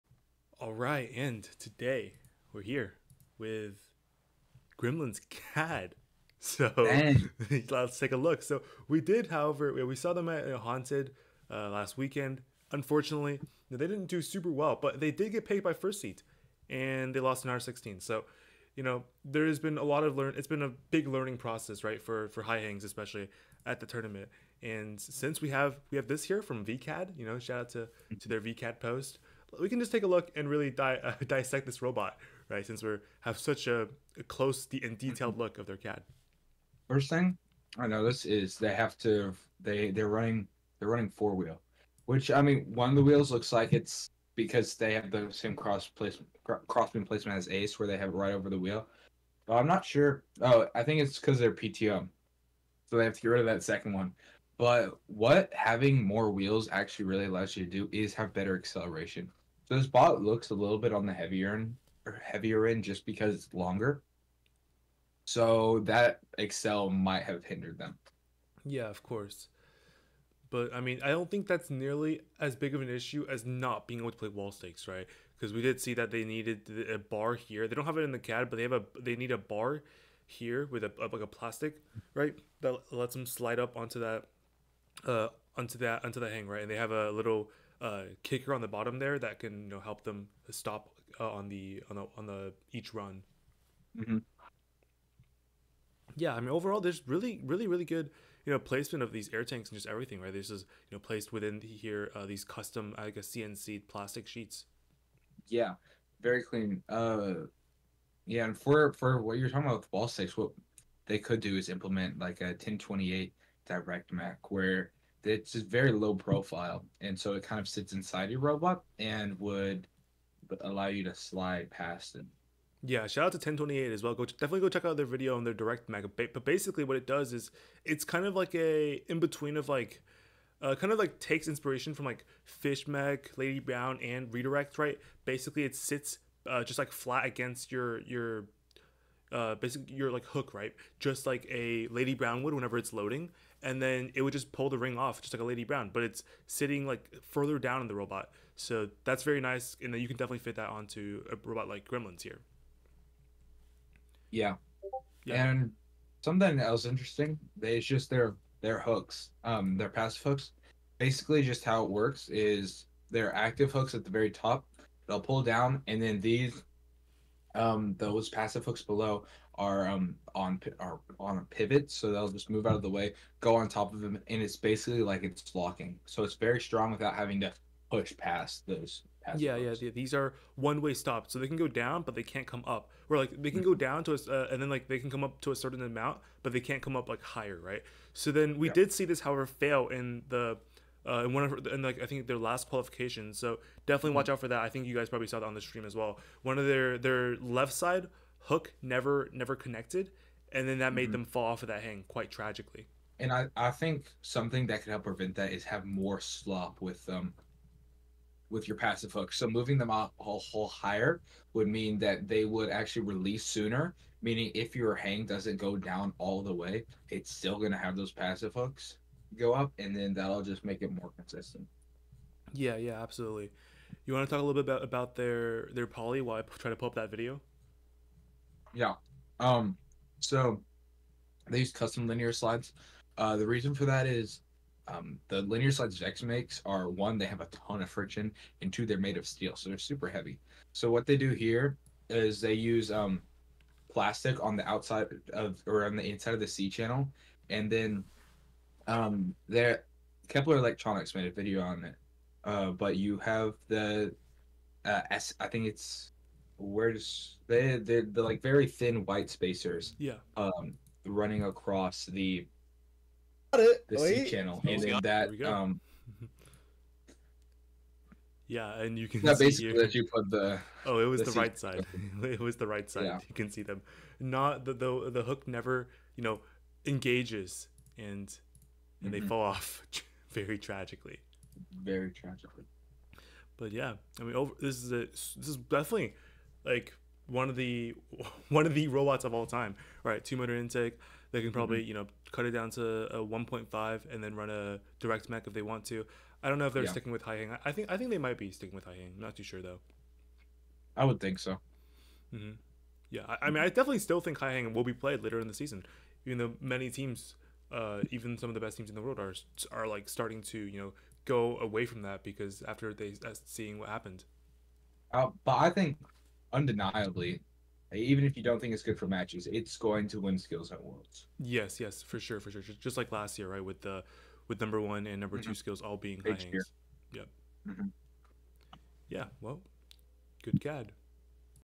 High Hang Cad...
today me and liam talk alittle bit about the 5203G high hang cad that they posted in Vcad.

VEX V5 Robotics Competition High Stakes is a game where two alliances compete to score more points than the other:

Field: A 12' x 12' square field with a ladder, stakes, mobile goals, and rings

Alliances: Two alliances, one red and one blue, each made up of two teams

Objective: Score more points than the opposing alliance by scoring rings on stakes, placing mobile goals, and climbing the ladder

Scoring: Each ring scored on a stake is worth one point, and the top ring on each stake is worth three points

Mobile goals: Hexagonal scoring objects with a maximum diameter of 10 inches and an overall height of 14.5 inches

Ladder: Located in the center of the field, with three levels and one high stake

Match: Consists of a 15-second autonomous period followed by a 1 minute and 45-second driver controlled period

Rules: Rings can only be counted for points once, and you cannot touch scored rings or mobile goals in the positive corners during the last 15 seconds of the match